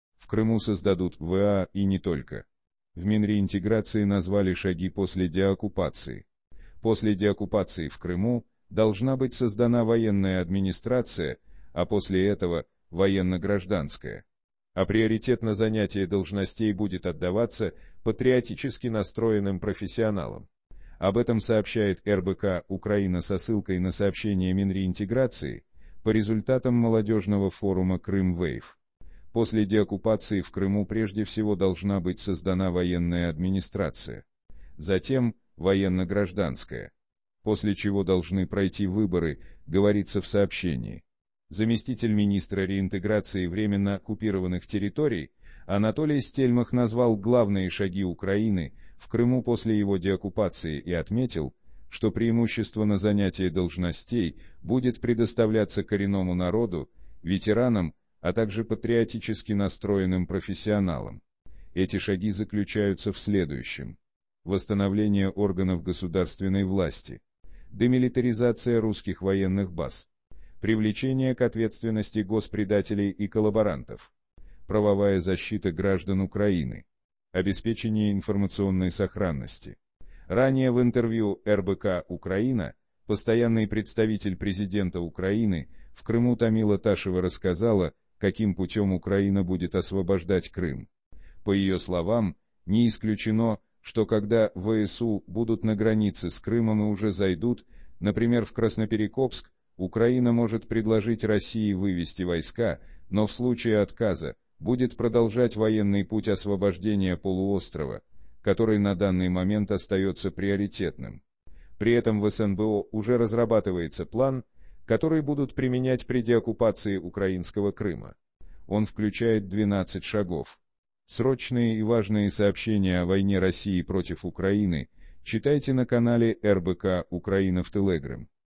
В Крыму создадут ВА и не только. В Минреинтеграции назвали шаги после деоокупации.
Крим После деоккупации в Крыму должна быть создана военная администрация, а после этого - военно-гражданская. А приоритет на занятие должностей будет отдаваться "патриотически настроенным профессионалам". Об этом сообщает РБК-Украина со ссылкой на сообщение Минреинтеграции по результатам молодежного форума "Крым Wave". "После деоккупации в Крыму, прежде всего, должна быть создана военная администрация. Затем - военно-гражданская. После чего должны пройти выборы", - говорится в сообщении. Заместитель мин